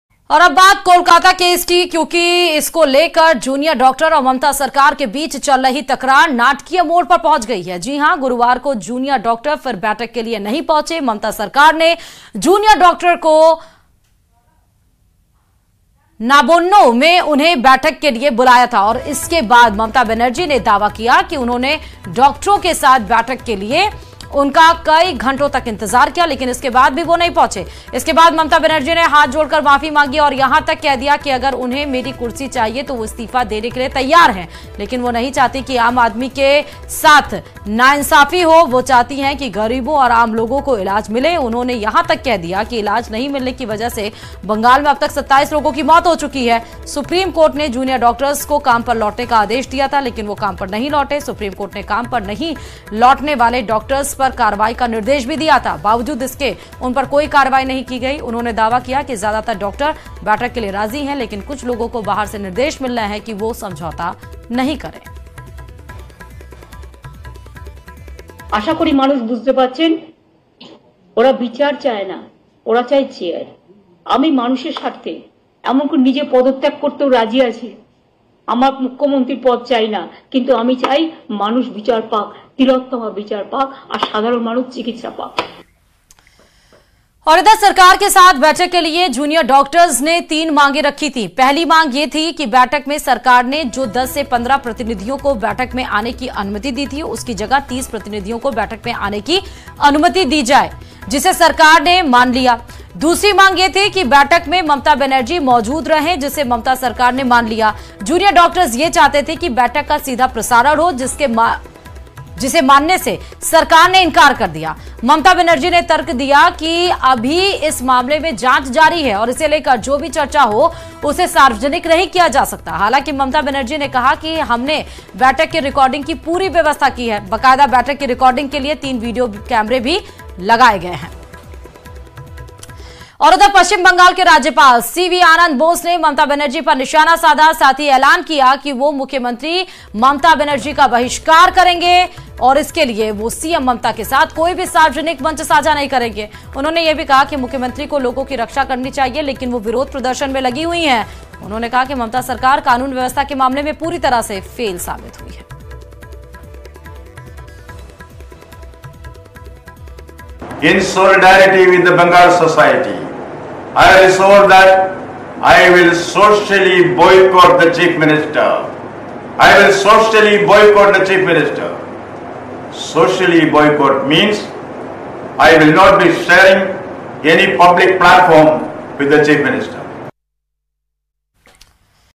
Mamata Banerjee Resignation News : 'नहीं चाहिए कुर्सी, इस्तीफा देने को तैयार' Kolkata Doctor Case
Kolkata Doctor Case को लेकर करीब 1 महीने से Junior Doctors Protest कर रहे हैं.  इसी बीच Mamata Banerjee ने अपने इस्तीफे को लेकर बड़ा बयान दिया है.  सुनिए क्या बोलीं Mamata Banerjee...